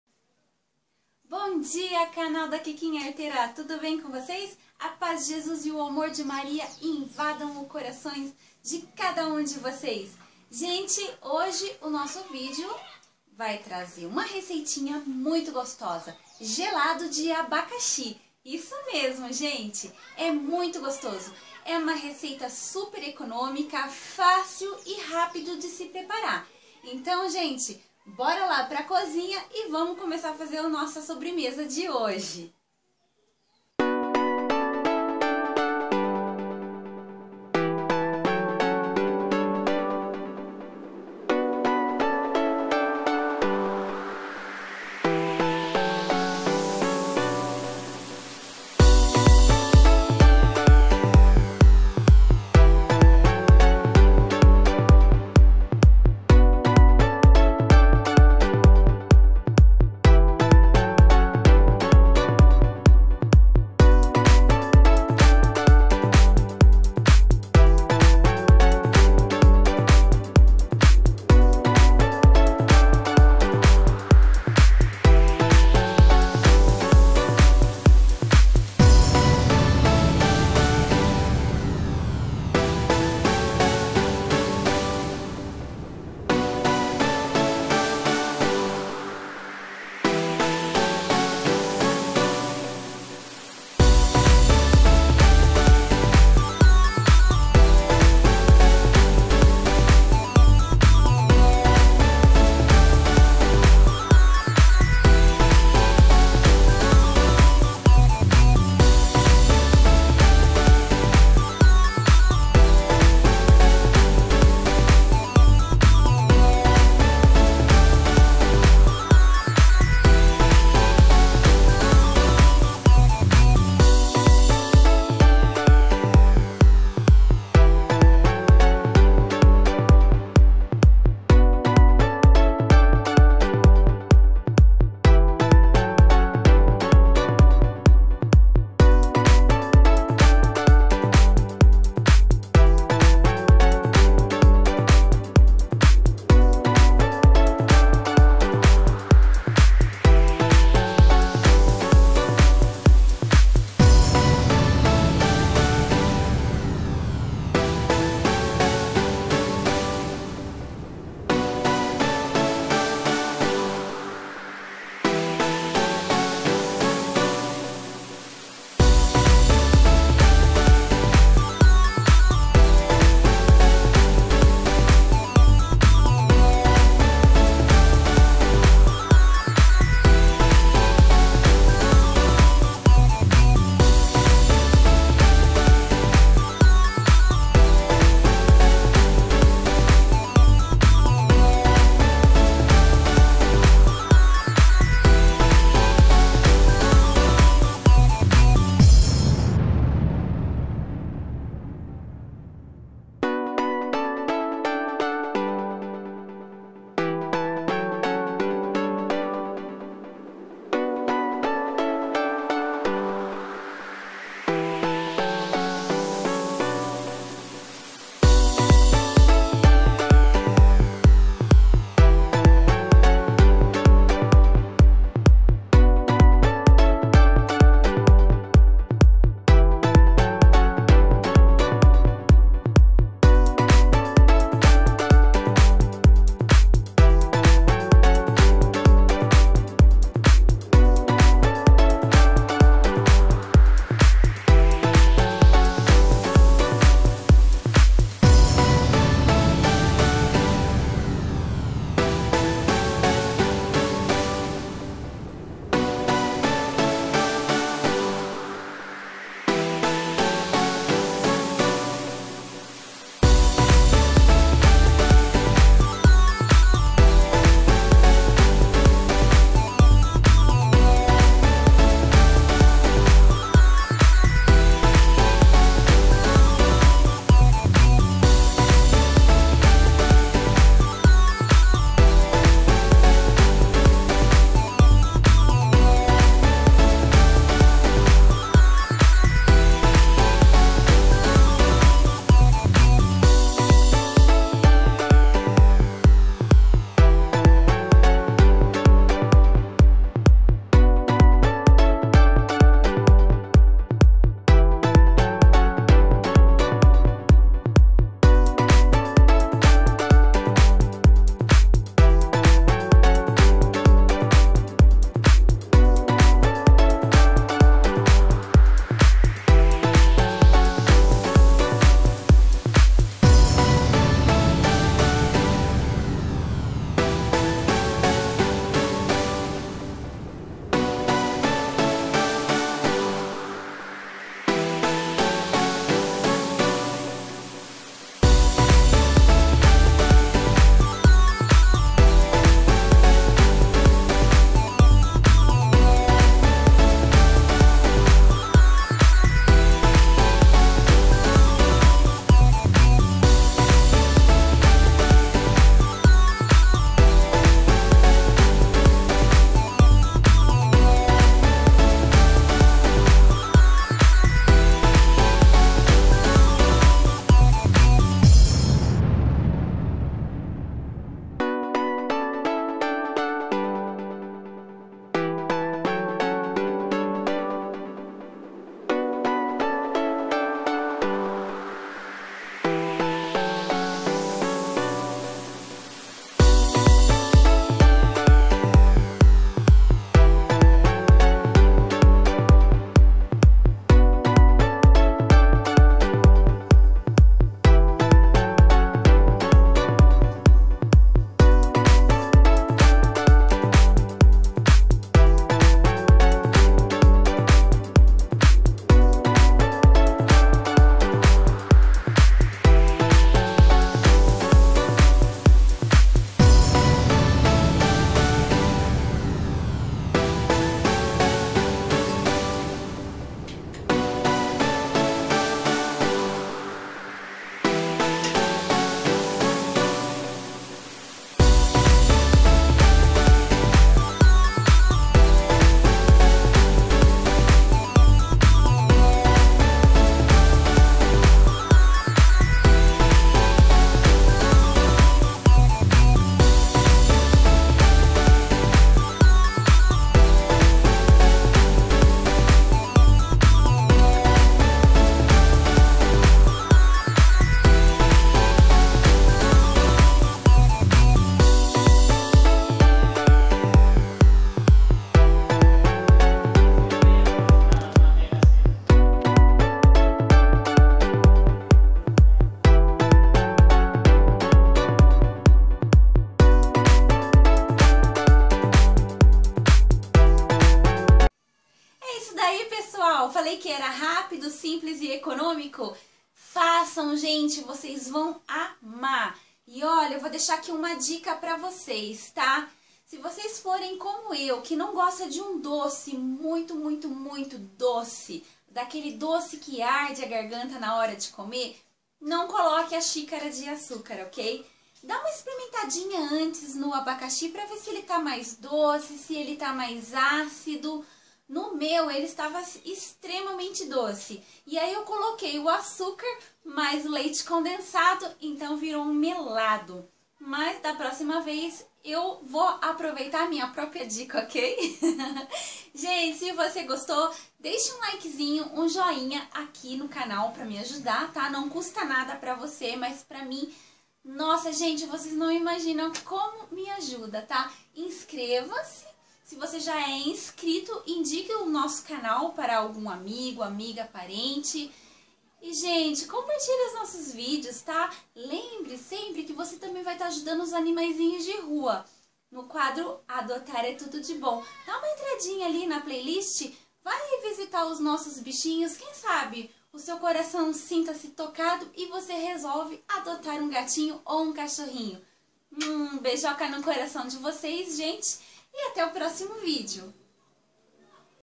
Gelado de Abacaxi,com 10,00 reais você faz uma deliciosa sobremesa bem cremosa
Essa sobremesa gelada com abacaxi e gelatina é deliciosa e fácil de fazer!
Olá, venham aprender uma receita deliciosa e prática de gelado de abacaxi, qualquer dúvida só deixar aqui nos comentários. Espero que gostem da receita, beijos e fiquem com Deus! ;*

✉ Contato Kikinha Arteira​:
(Anúncios/Parcerias/Negócios)]

falando um pouquinho de mim